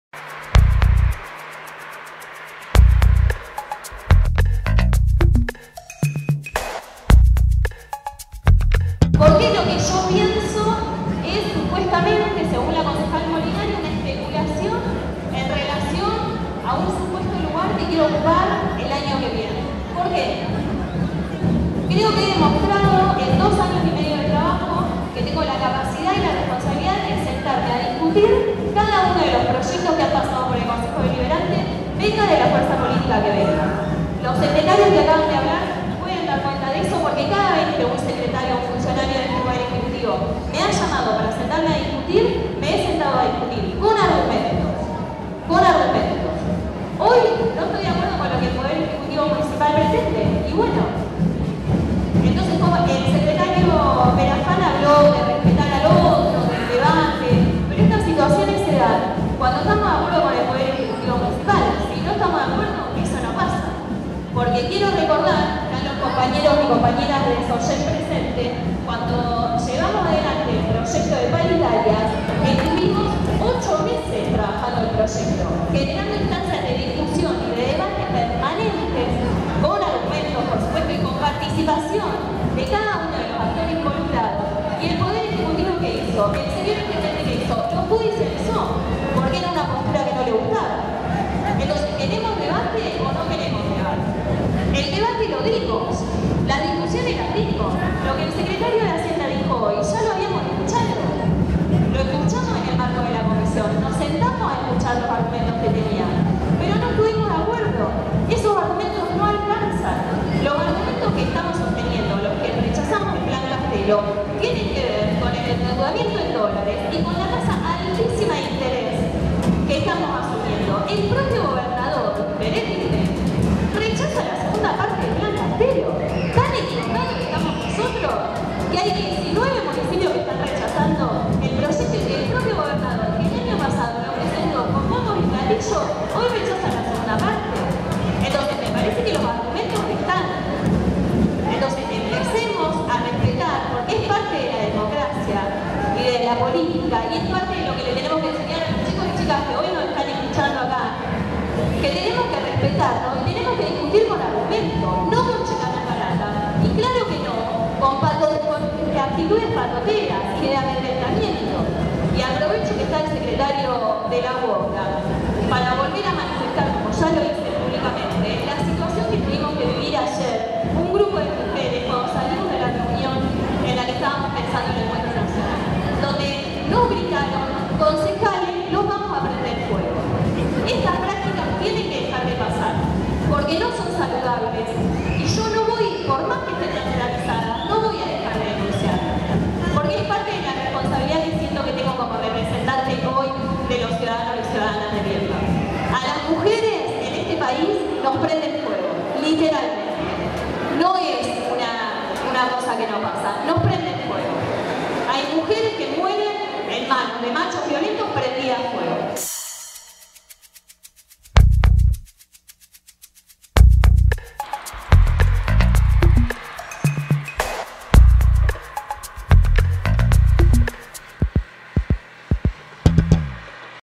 MARIANA ARREGUI - DEBATE PLAN CASTELLO
22/05/18 MÓVIL EN VIVO de Radio Encuentro y entv con extensos tramos de la sesión que se desarrolló hoy en la ESFA.

El Concejo Deliberante no autorizó la deuda en dólares que el intendente Foulkes quería para poder asfaltar varias calles de la ciudad.  La UOCRA concurrió a la sesión que se realizó en la ESFA, la escuela donde funciona la radio El Refugio, recientemente declarada de interés Cultural, Social y Educativa.  Representantes de la UOCRA se mostraron muy molestos con el tratamiento del tema que interpretan los afecta directamente porque las obras garantizarían trabajo a obreros del sector. Luego de la votación un grupo agredió a la concejala Silbana Cullumilla. La sesión debió interrumpirse y se terminó abruptamente.